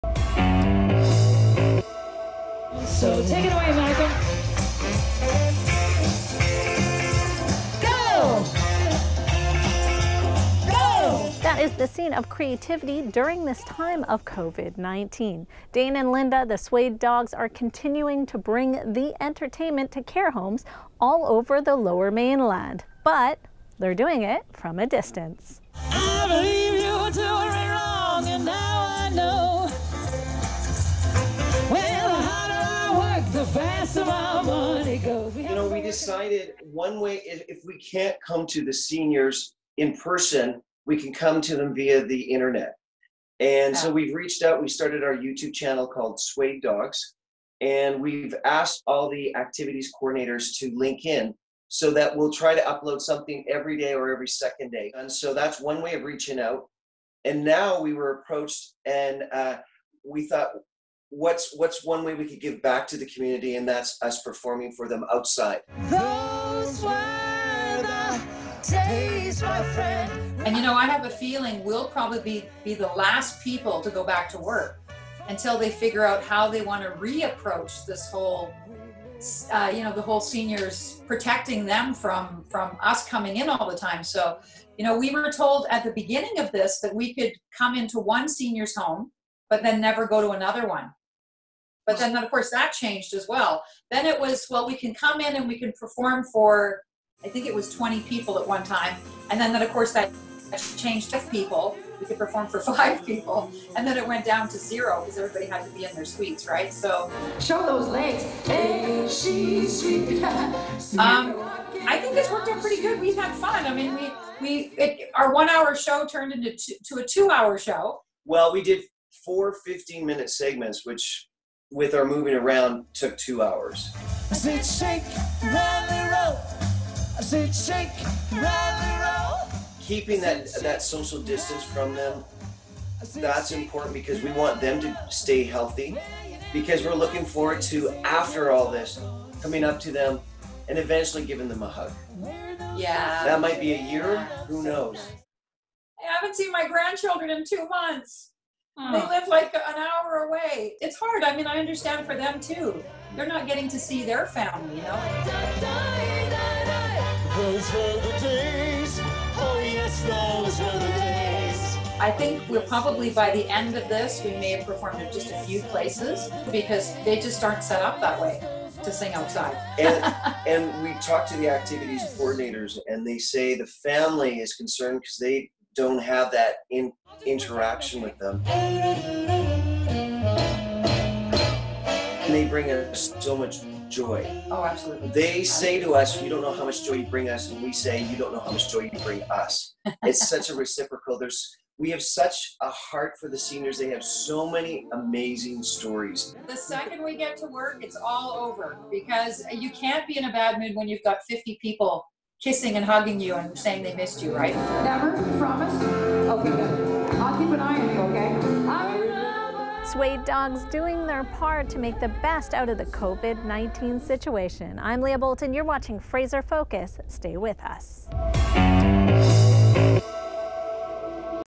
Fraser Focus - COVID-19 Care Home Entertainment
Seniors all over Canada and the world are being isolated in care homes due to COVID-19.  Local entertainers, Dane and Linda, have found a creative way to stop by and entertain while following the physical distancing rules.  Leah Bolton takes a look...